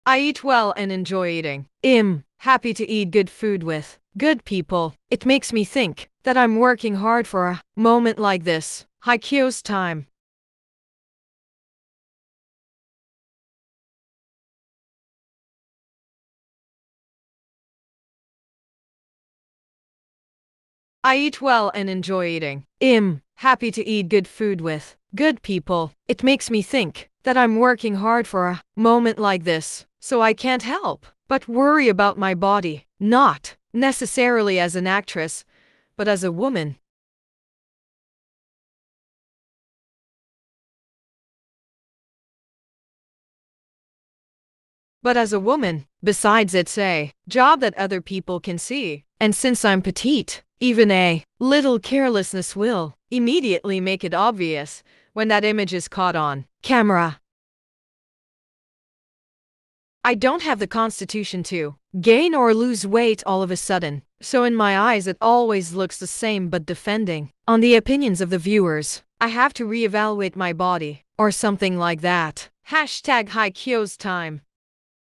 hye Kyos time I eat well and enjoy eating. im happy to eat good food with good people. #2022
hye Kyos time I eat well and enjoy eating. im happy to eat good food with good people. #2022 song hye kyo